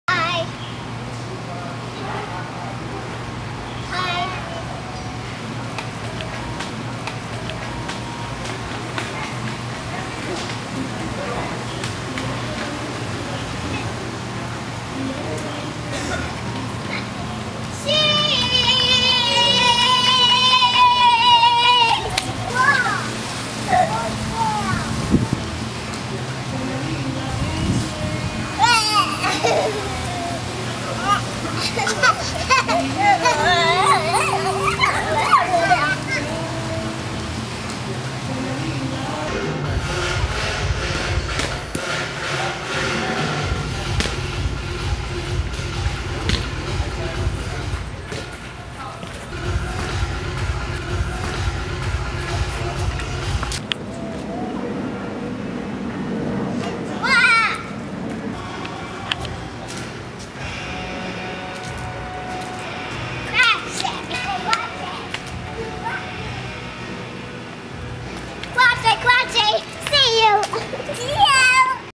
CUTTEST Little Kids Play Bocce Ball Game! Milana and Andre Sky.
Milana and her toddler brother are having fun playing Bocce Ball Game. Super Cute!

Special thanks to: Anastasia Sky Photography for creating this video.